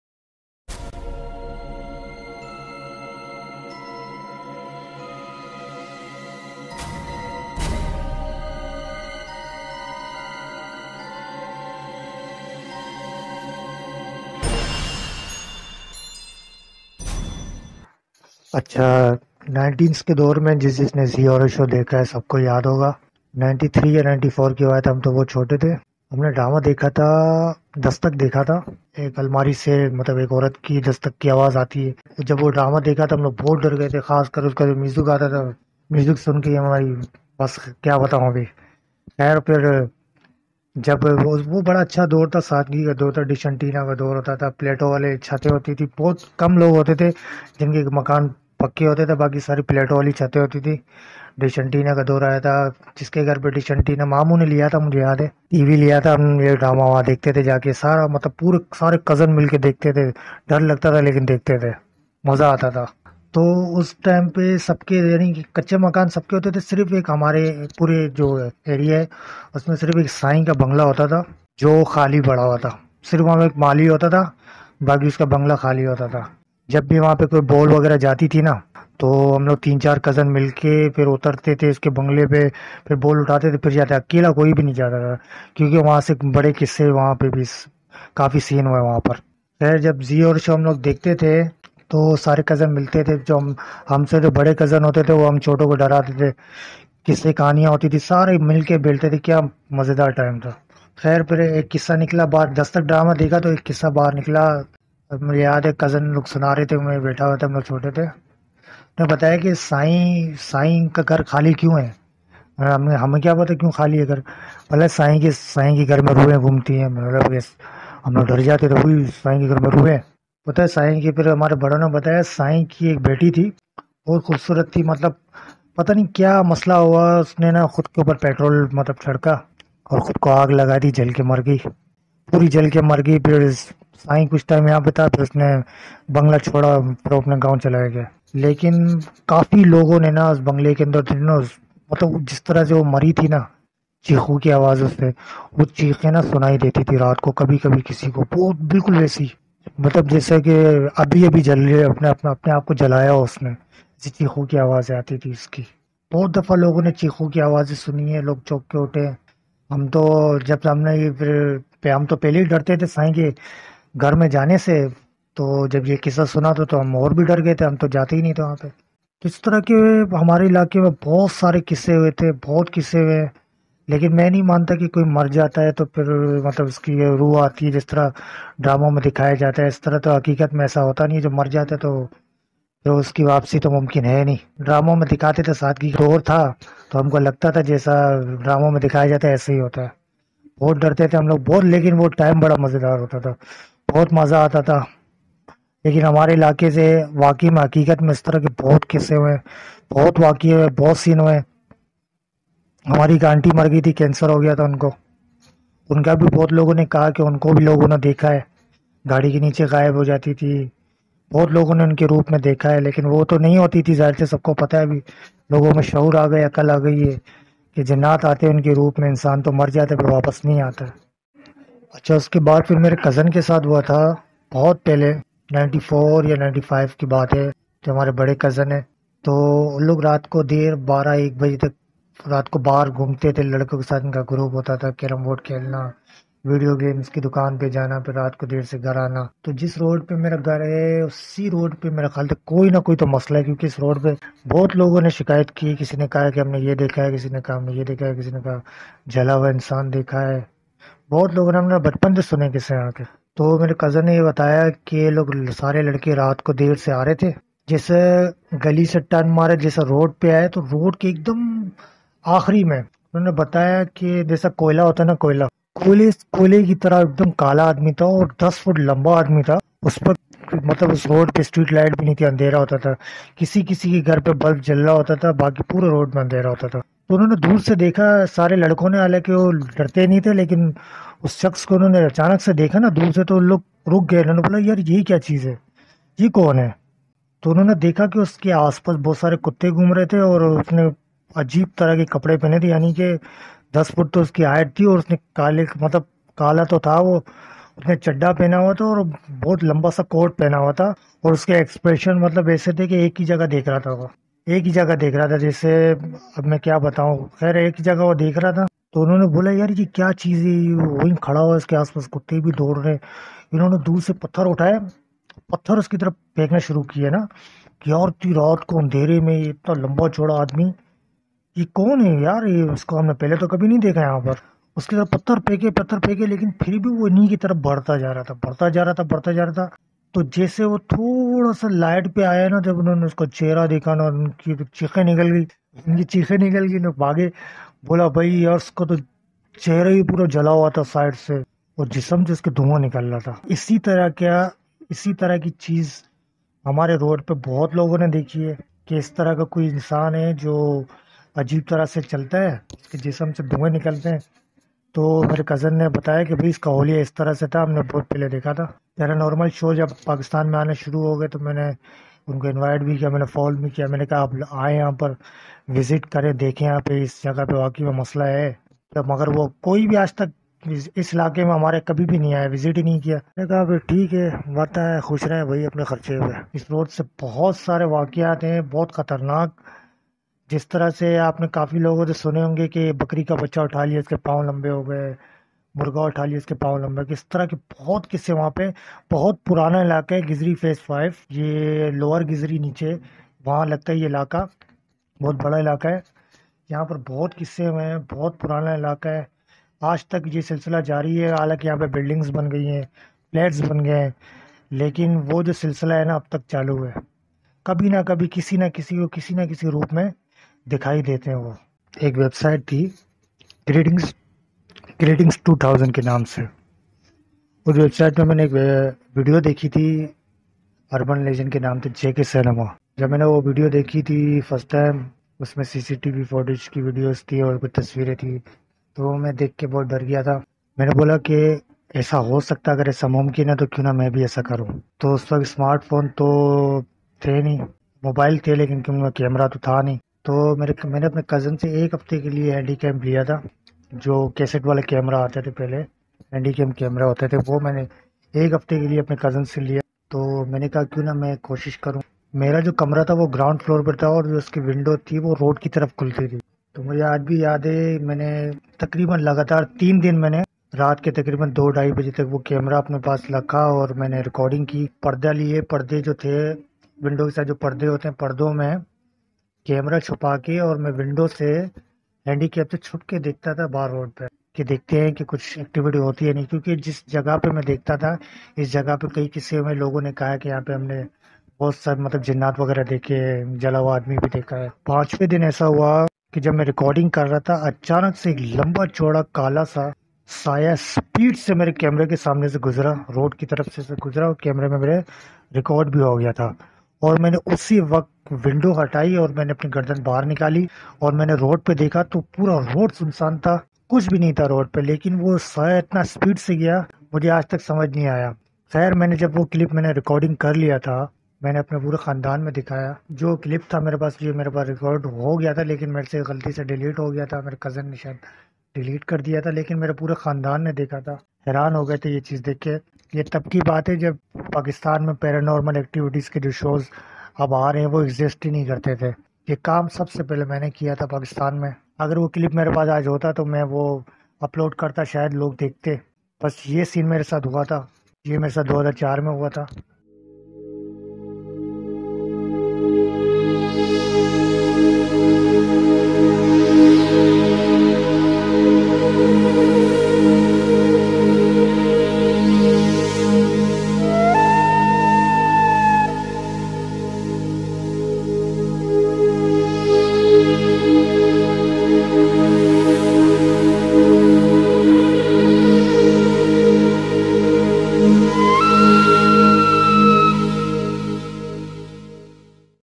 Urdu Horror stories | Andheri Rat Kala lamba Jinn | Scary stories Aap Beetiya | Horror Tapes
3 khaufnak Suchi Kahaniyan Purasirar ghar dil dehla dene wale waqyat such per mabni kahaniyan